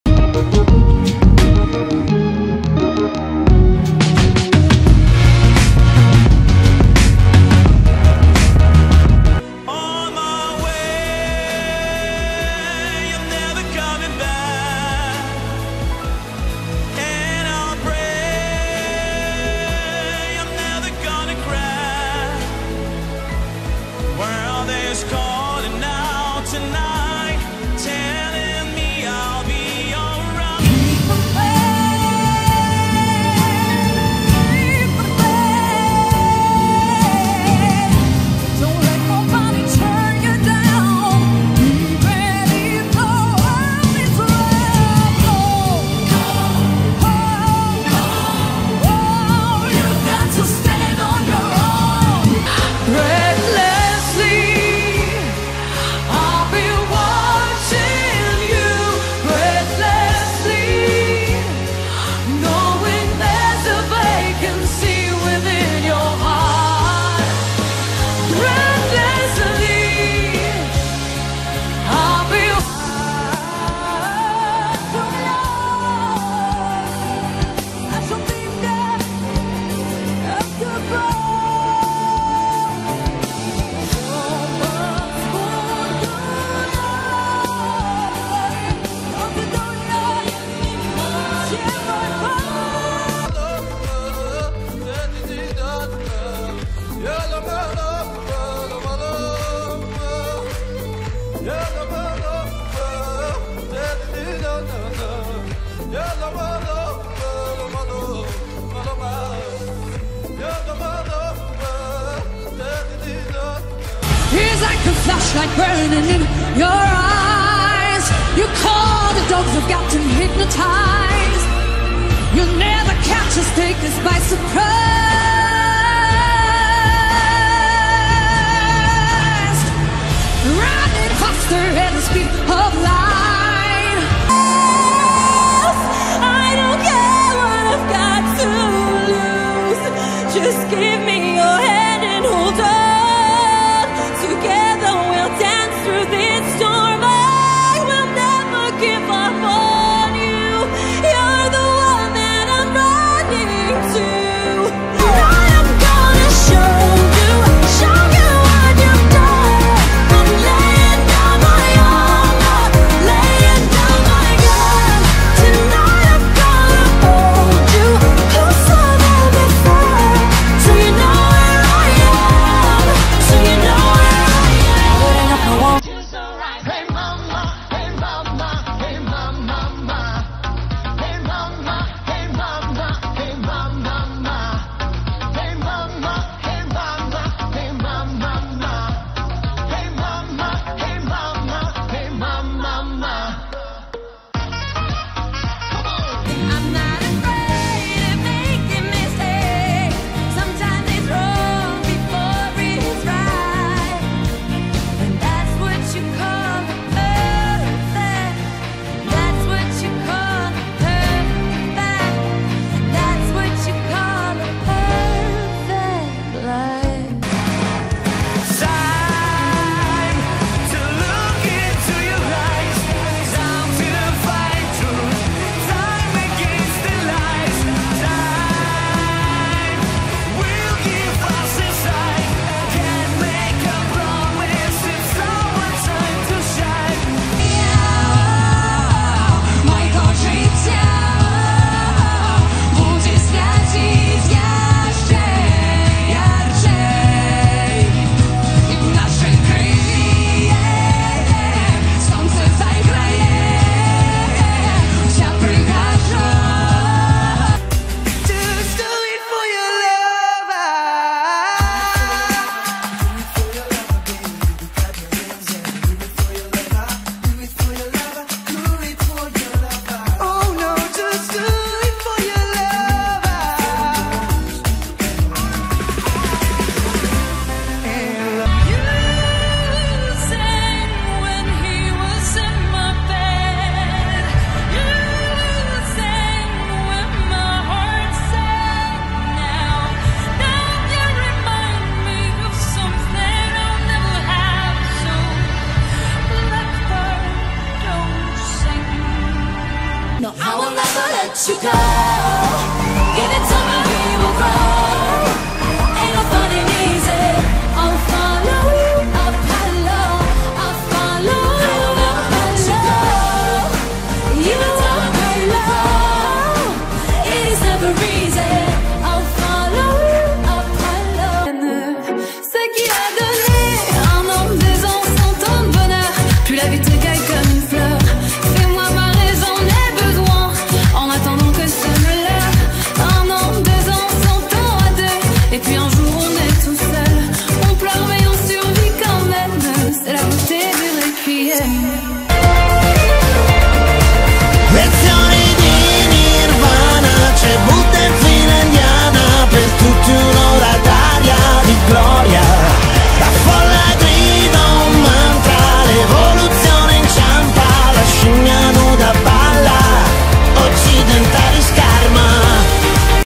TOP 17 | EUROVISION 2017 (ESC 2017) so far
Welcome to many countries!
This is my top 17 of Eurovision Song Contest 2017
I welcome your comments
Greetings from Chile :)